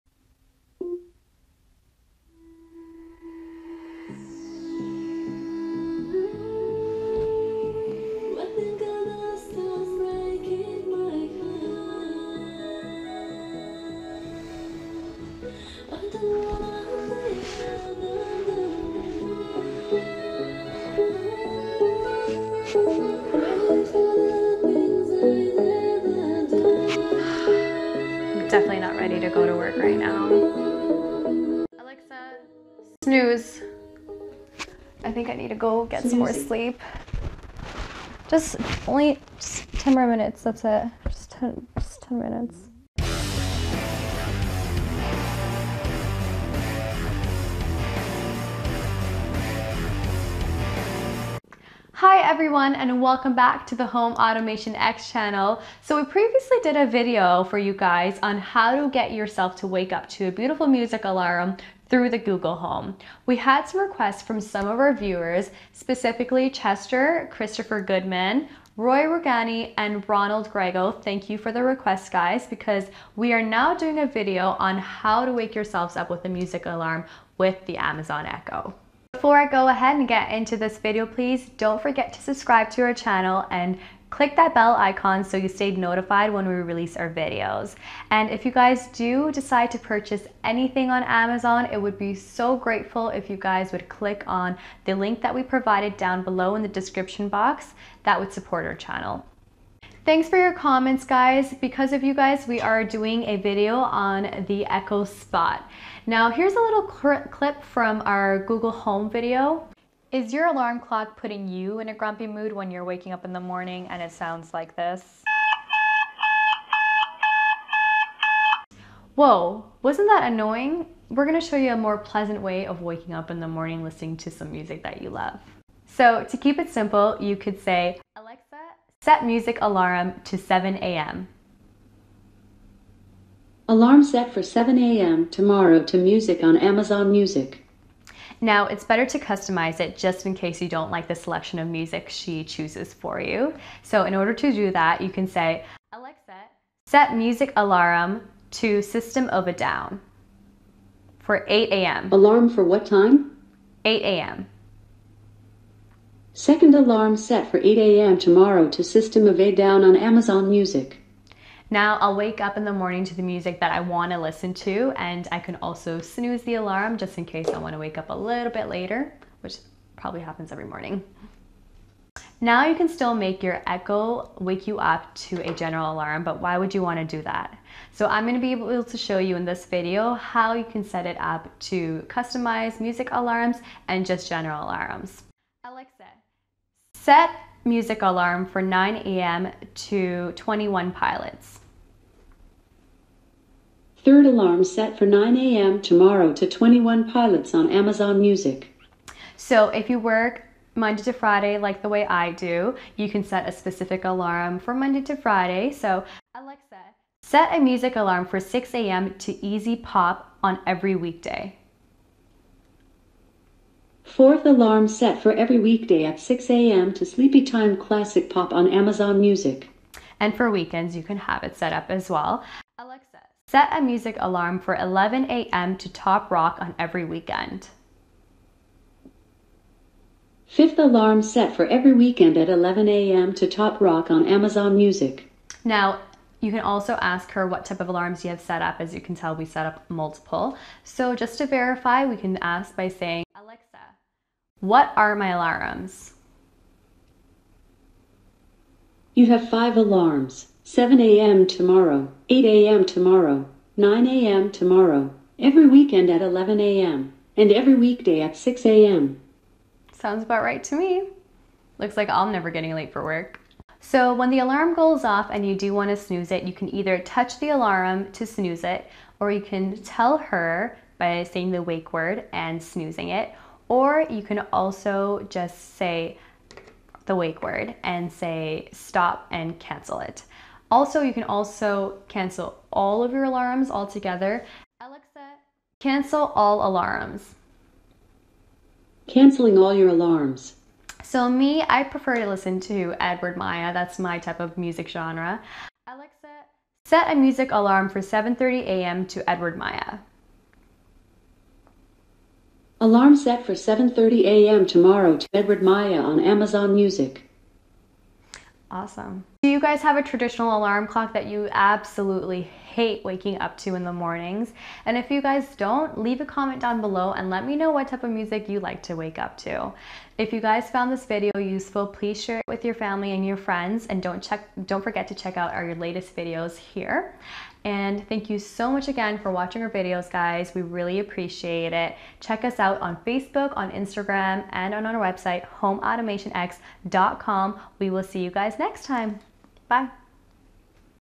Amazon Echo Alexa Music Alarm Clock Feature
Wake Up to Music with Your Amazon Echo with the alarm feature. This video will show you how to set music alarms with your Amazon Echo Spot. Wake up in peace with your Amazon Echo Alarm.

Our Smart Home Tech Shop (USA)
Smart Home Tech (International)

Now back to how you can set music alarms with the Amazon Echo. This will also work with the Echo Dot. Our review walks you through the New Amazon Echo music alarm feature.

This alarm feature can play music or radio on your Amazon Echo. We show you what you need to know to set alarms on your Eco Spot. There are many Amazon Echo Alarm commands.

Amazon Echo plays music, and now the Echo has music alarms. We review how to set Home Music Alarm.